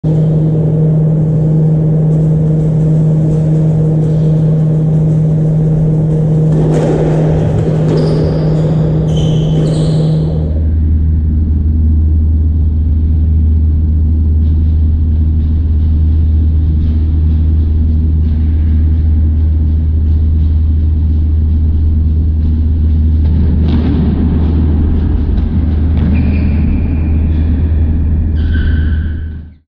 10 Balls 36 Catches | 2024 NJL
A nice run of 10 from my first session of the year :) A new PB soon I reckon.